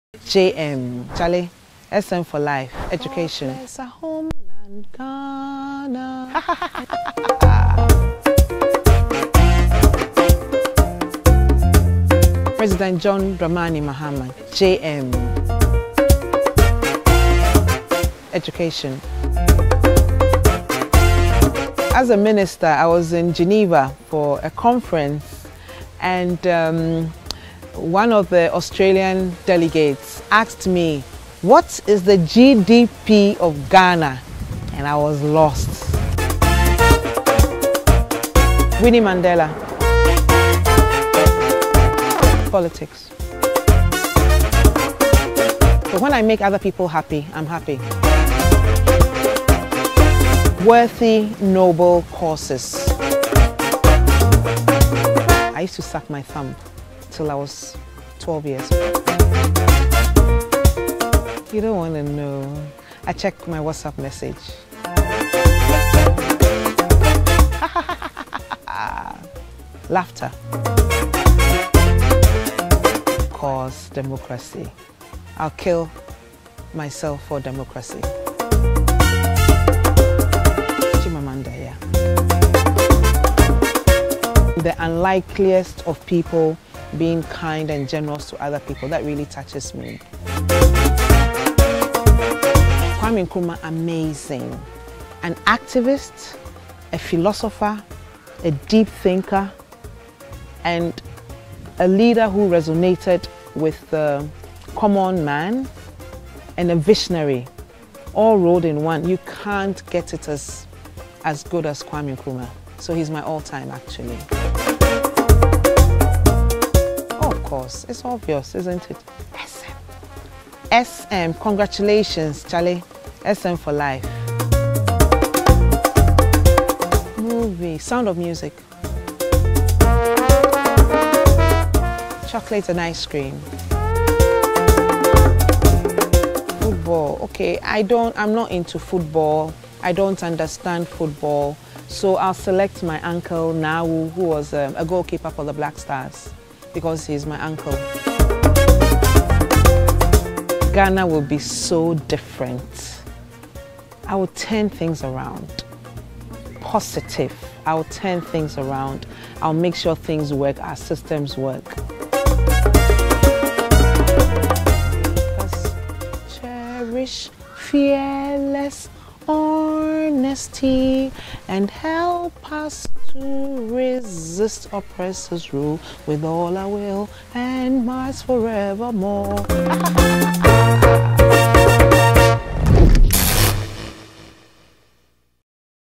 I Will Kill Myself For Democracy. 20 Interesting facts about Nana Oye Lithur.
Nana Oye Lithur's most embarrassing moment was when she was asked Ghana's GDP and she did not know.
Find out 20 facts about Nana Oye Lithur.
She will kill herself for democracy.